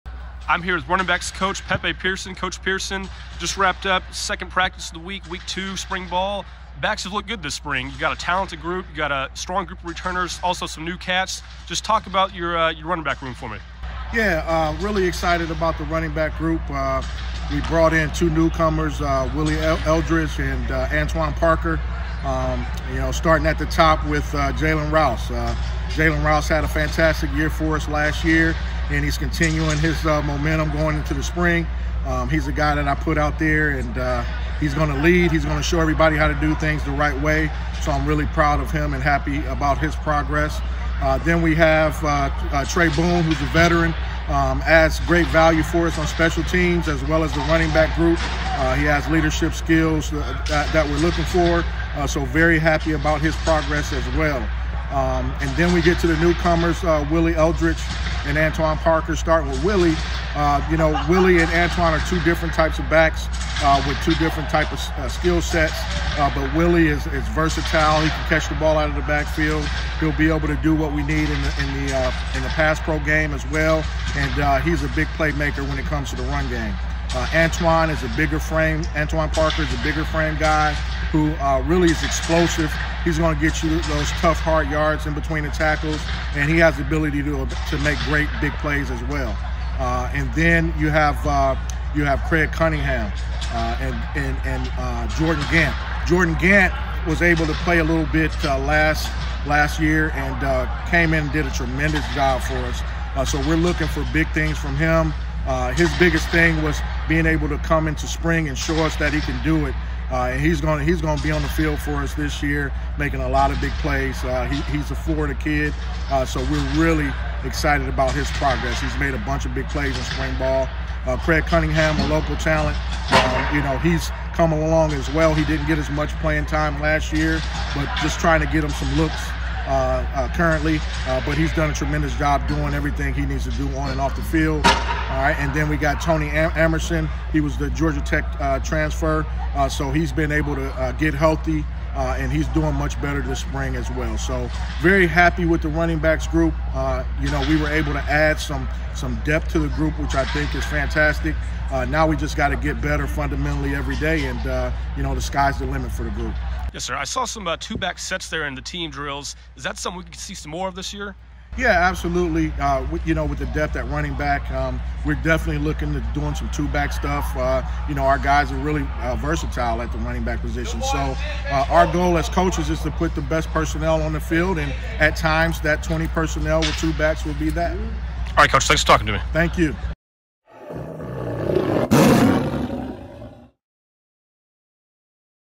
FB | Pepe Pearson Post Practice Presser (March 29, 2023)
Hear from Running Backs coach Pepe Pearson after the team wrapped up spring practice No. 5. Pearson breaks down the RB room and talks about a few of the new signees and some of the key returners to this year's squad.